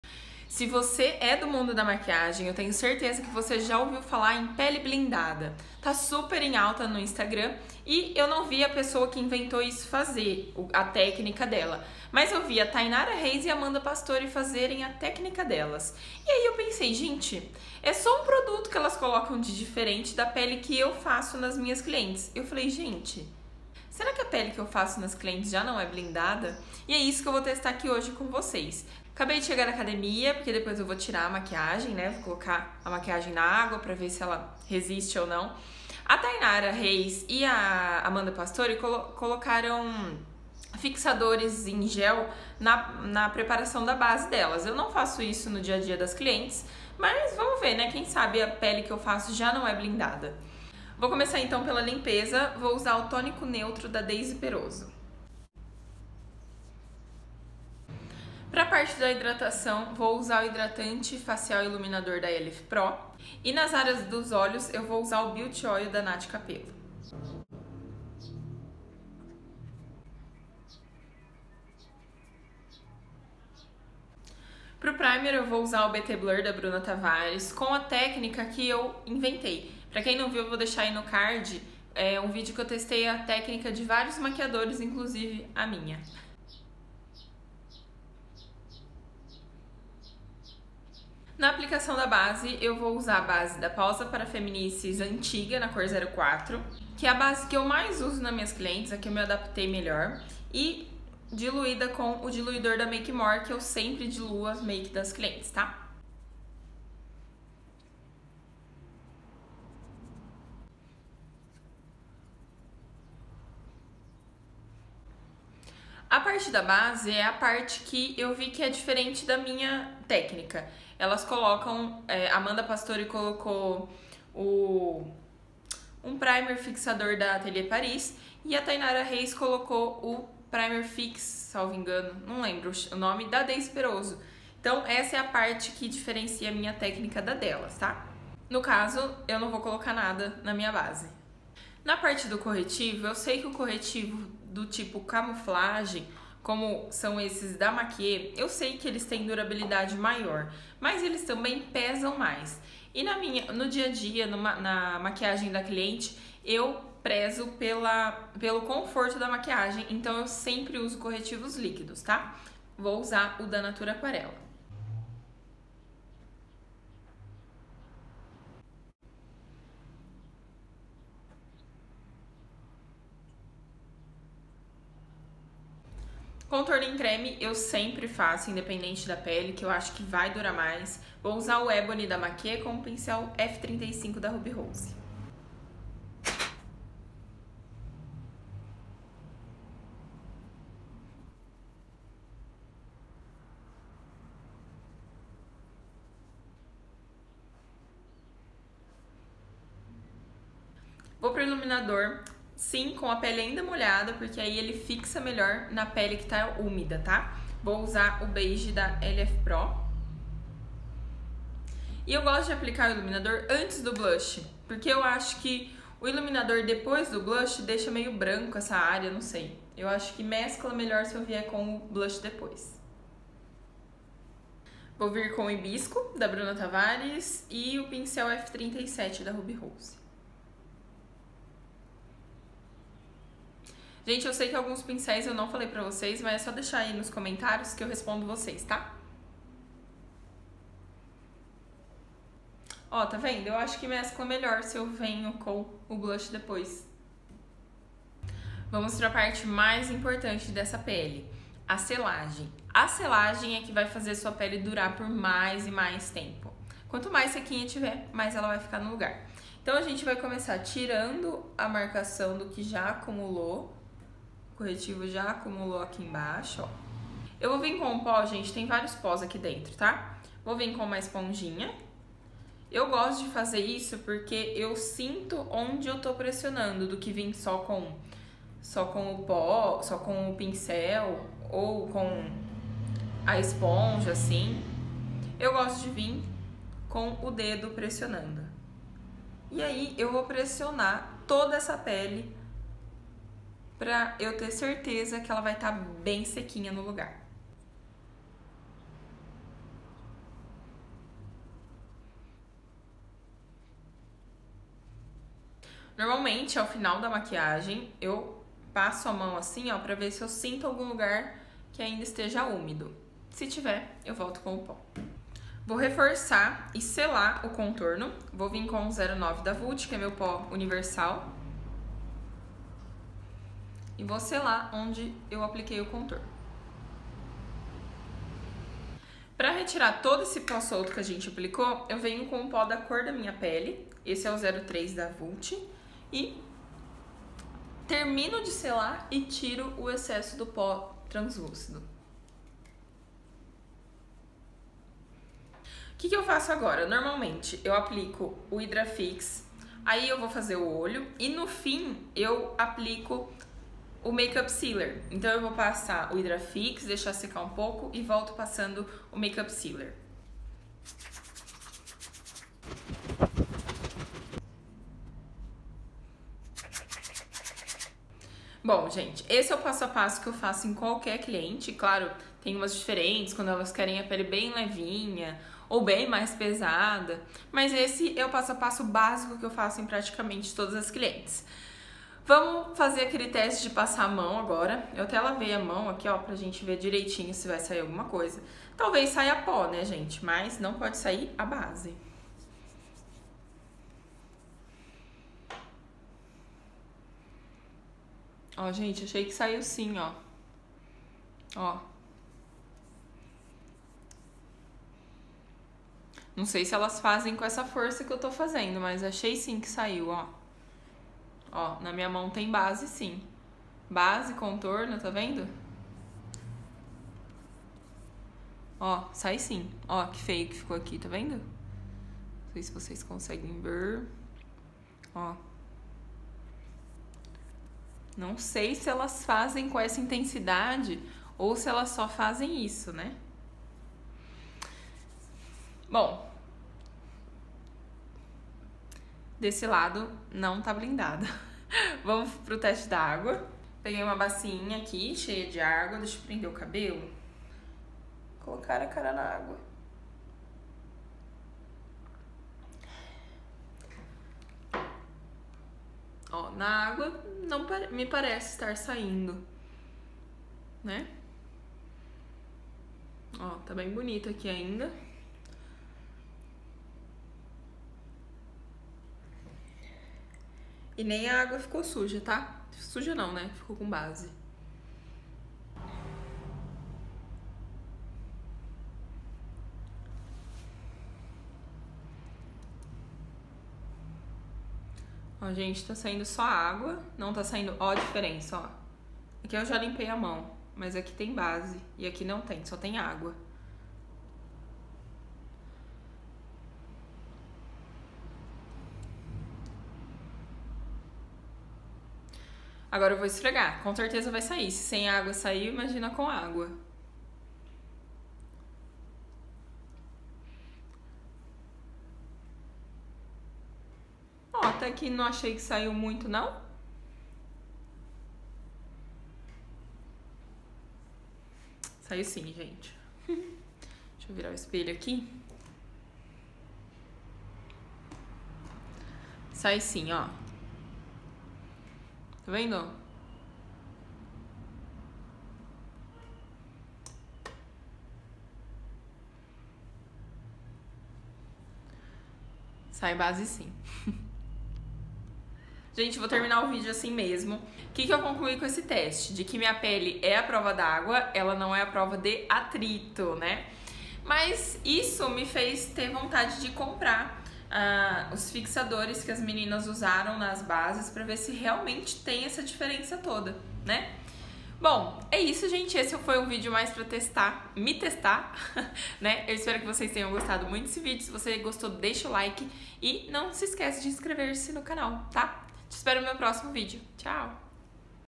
PELE BLINDADA COISA NENHUMA
descrição dos produtos para melhor acessibilidade:
tonico neutro deisy perozo
hidratante iluminador lfpro
beautyoil nath capelo
btblur
pausa 04 ppf
diluidor makemore
corretivo natura aquarela
contorno ebony makie, f35 rubyrose
iluminador beige lfpro
hibisco blush bt
pó translucido várias marcas
09 vult contorno em pó
pó compacto 03 vult
hidrafix deisy perozo
makeup sealer deisy perozo